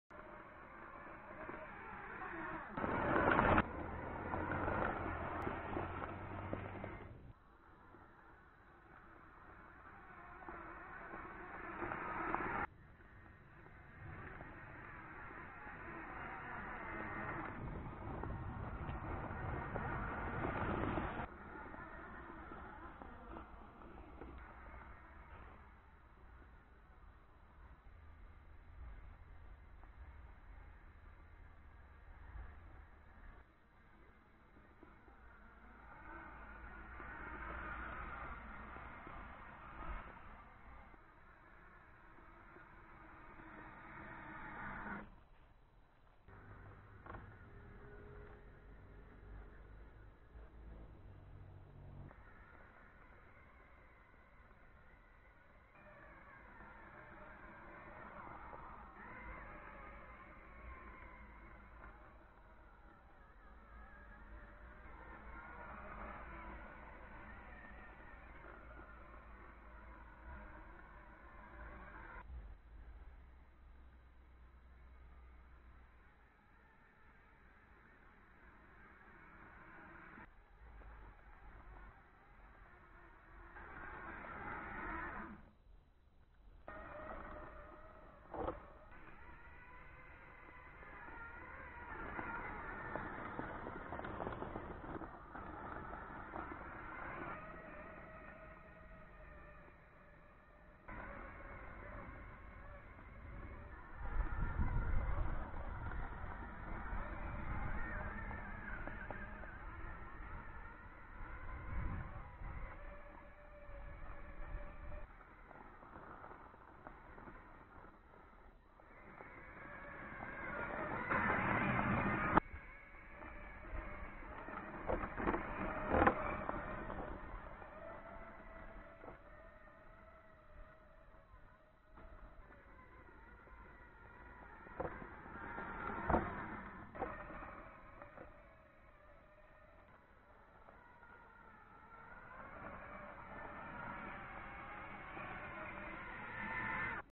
6WD Roboter Schildkröte im Wald
6 Reifen, 6 Motoren, 6 Getriebe, Einzelradaufhängung, mehr Geländegängigkeit ist kaum möglich. Dieses Fahrzeug ist zur Zeit ein getarnter Grashügel, soll aber bald mit einer DSLR-Kamera ausgerüstet werden für autonome Tierbeobachtung